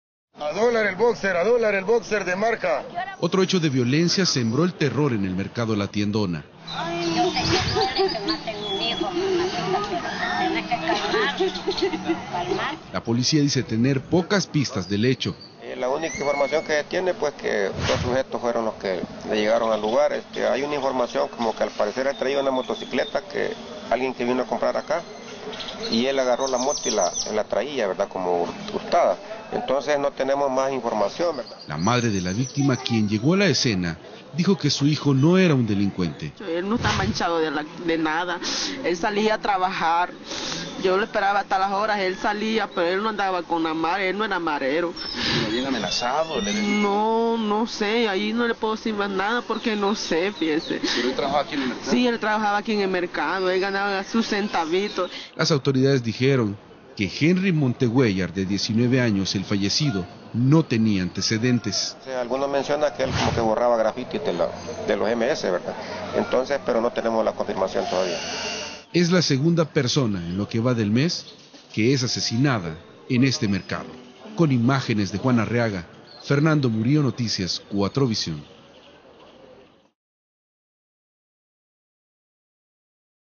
Asesinato en La Tiendona.
Nuevamente corrió la sangre en el Mercado La Tiendona de S.S. La víctima era un trabajador del lugar. Este nuevo asesinato aumentó el miedo de los comerciantes. (13/12/2013)